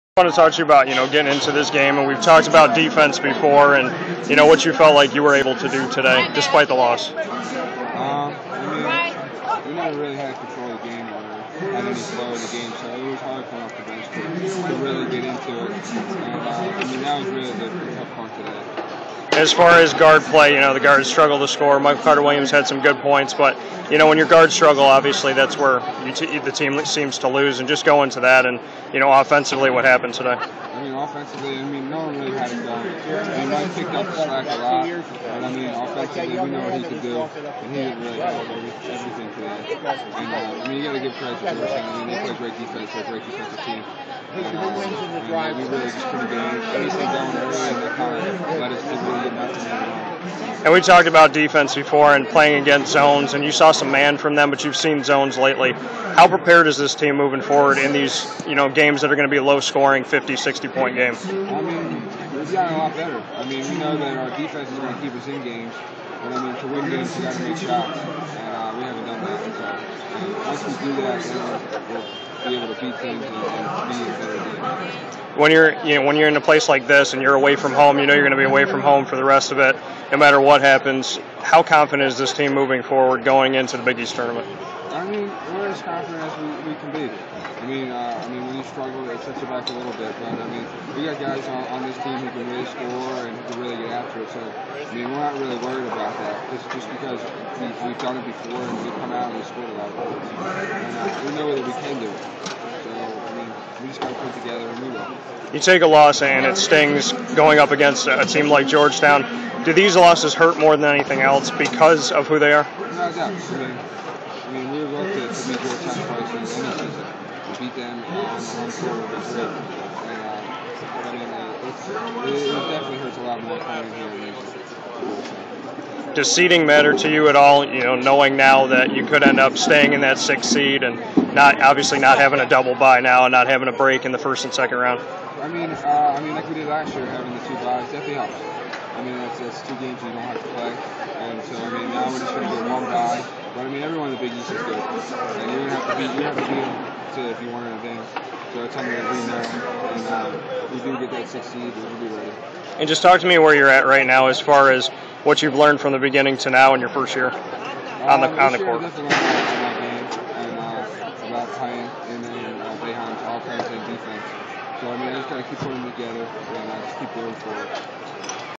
Trevor Cooney - Final Big East match w/ Georgetown
Redshirt-freshman of the Syracuse Orange gives his thoughts and feelings on the final Big East regular season contest with Georgetown, how the game went for Syracuse, what needs to be done, and more with Dan Tortora.